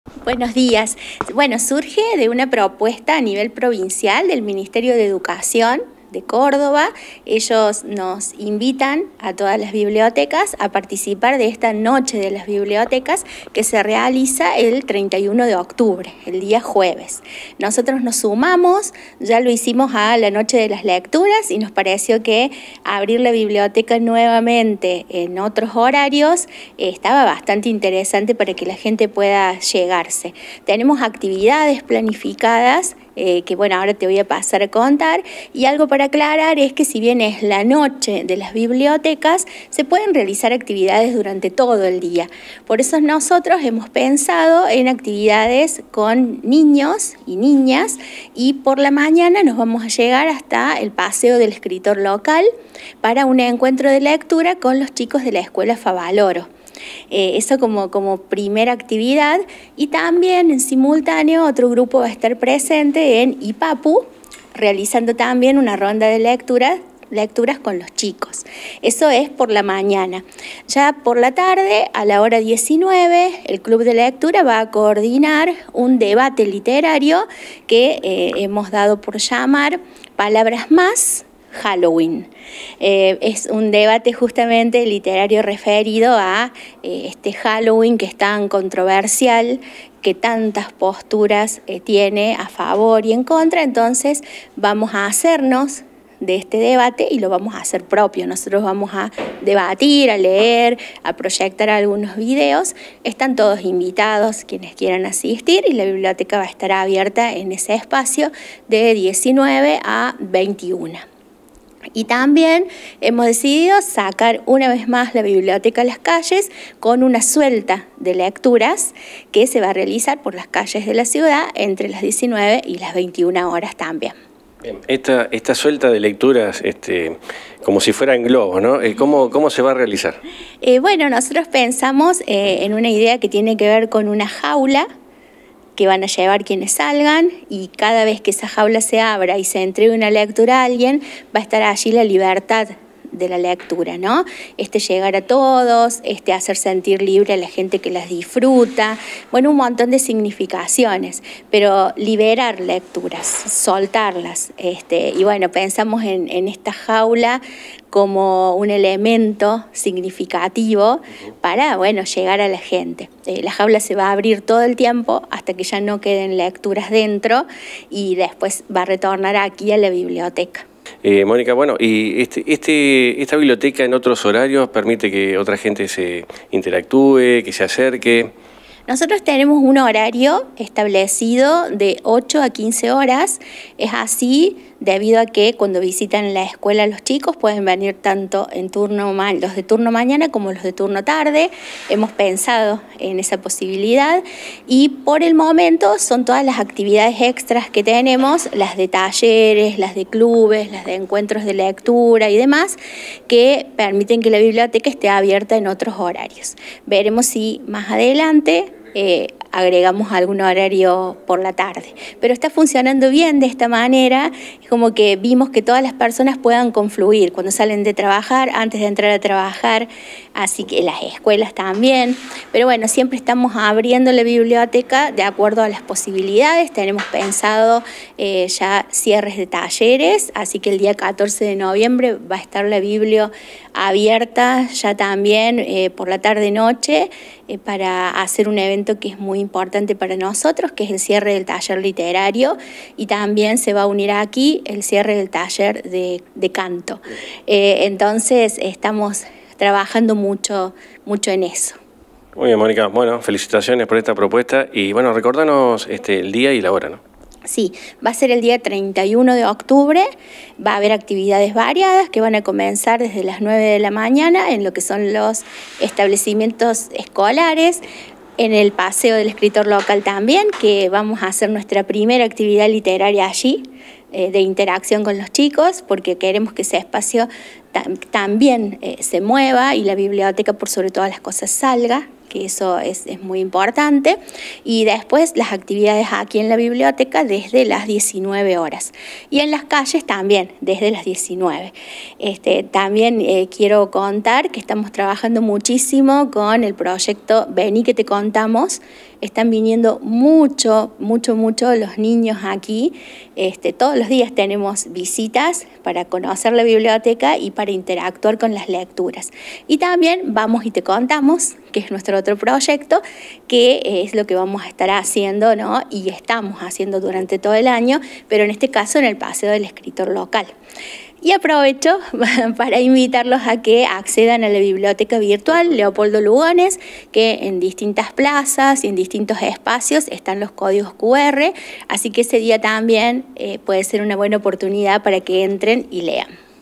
NOCHE DE LAS BIBLIOTECASJUEVES 31 DE OCTUBRE ACTIVIDADES DESDE LA BIBLIOTECA L.LUGONES
NOCHE DE LAS BIBLIOTECAS
JUEVES 31 DE OCTUBRE ACTIVIDADES
DESDE LA BIBLIOTECA L.LUGONES